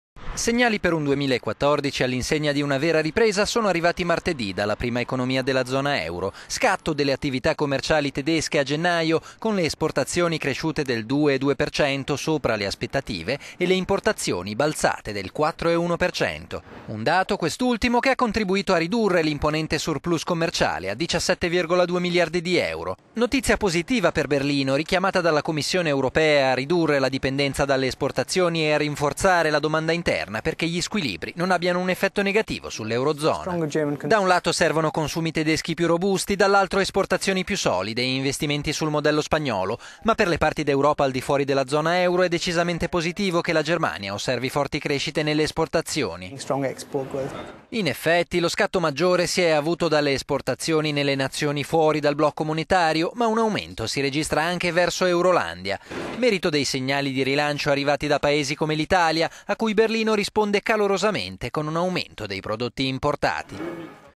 Germania, scatto di import ed export a gennaio - economy
Segnali per un 2014 all'insegna di una vera ripresa sono arrivati martedì dalla prima economia della zona euro.

Scatto delle attività commerciali tedesche a gennaio, con le esportazioni cresciute del 2,2%, sopra le aspettative, e le importazioni balzate del 4,1%.

Un dato, quest'ultimo, che ha contribuito ad ridurre l'imponente surplus commerciale a 17,2 miliardi di euro.

Notizia positiva per Berlino, richiamata dalla Commissione europea la settimana scorsa a ridurre la dipendenza dalle espor...